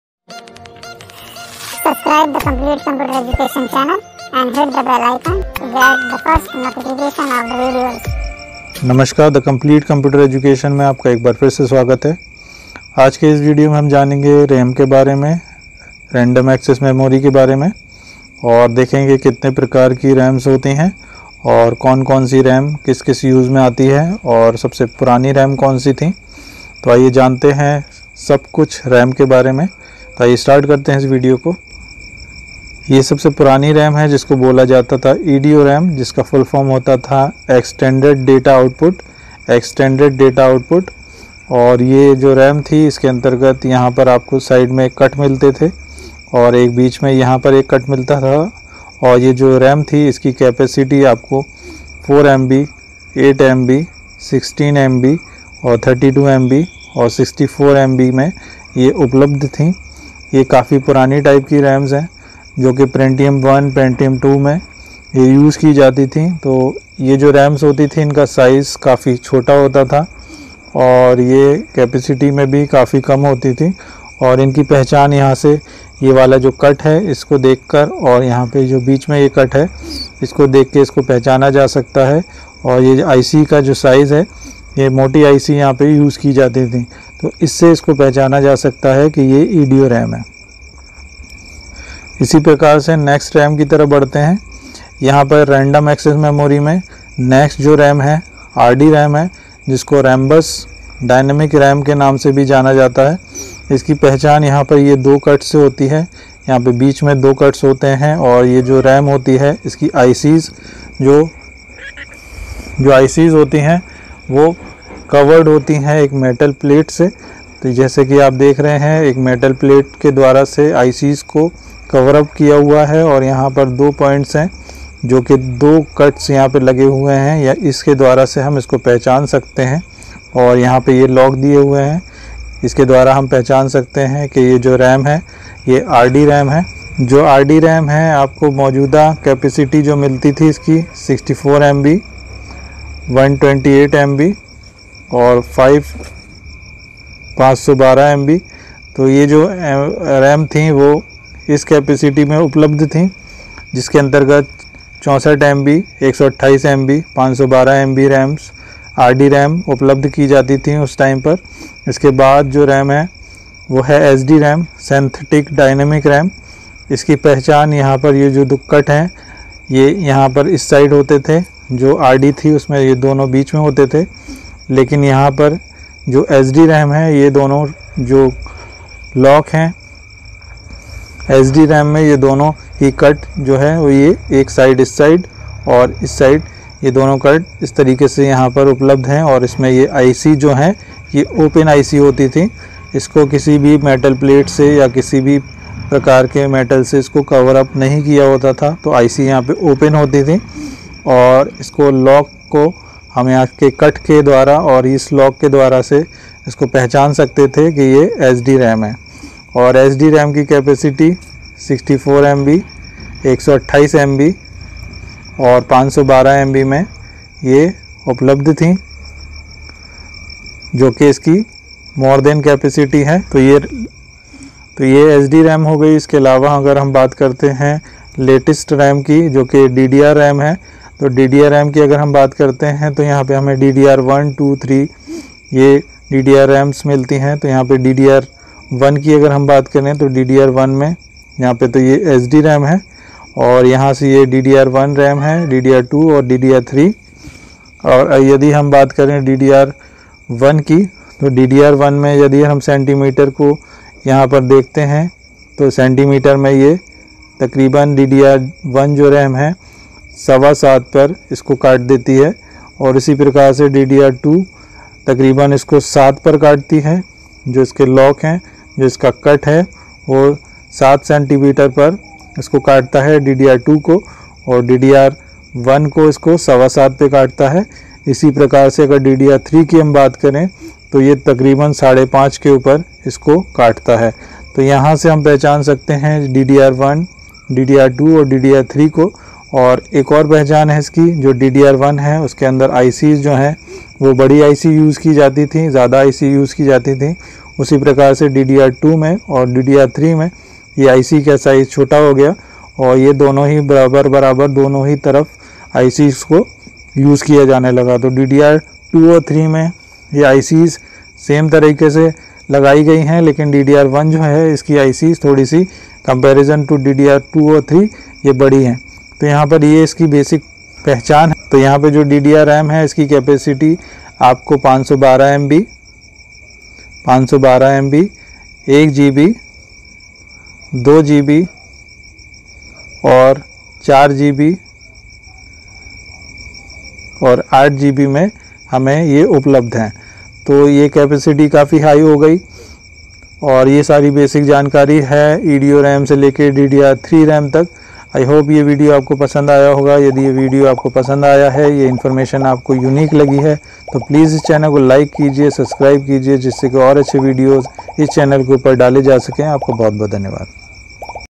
Types of Ram memory in computer | Edo Ram, RD Ram, SD Ram, DDR1, DDR2, DDR3, DDR4, DDR5
what is ram and how many Types of Ram memory in computer the Edo Ram, RD Ram, SD Ram, DDR1, DDR2, DDR3, DDR4, DDR5 all types of ram